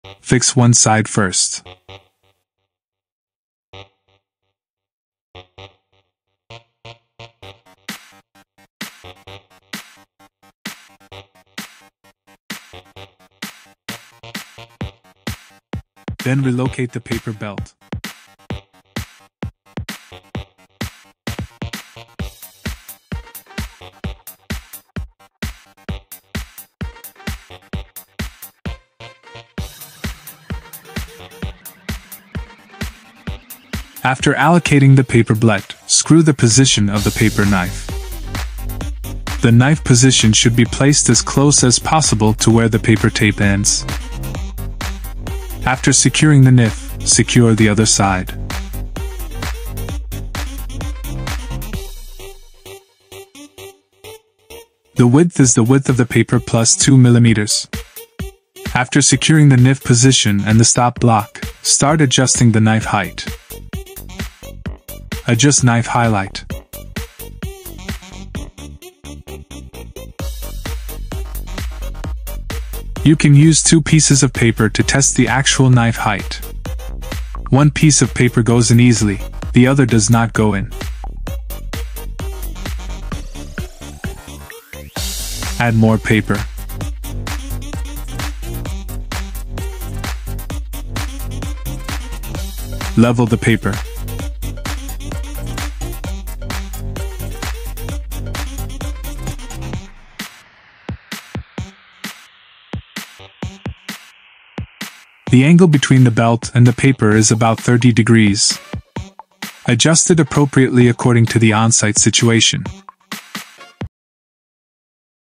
Episode 1-How to operate folder gluer machine. how to set up folder gluer machine paper knife
How to operate folder gluer machine. how to set up folder gluer machine paper knife
How to operate folder gluer machine.
XIESHUN folder gluer machine how to set up paper knife step:
1. Fix one side of the knife
2. Allocate the paper blet and secure initial knife in position
3. Secure the other side of knife
4. Set knife width to paper width plus 2mm
5. Adjust knife  height using paper test
6. Level the paper push
7. Set belt/paper angle around 30 degrees
8. Adjust paper as needed based on situation
(Look for distribution)
200~800g/㎡ cardboard and N/F/E flute corrugated board
Straight line,lock bottom,double sides,4&6 corner box, CD box
6 Corner box,set up Training,teaching video,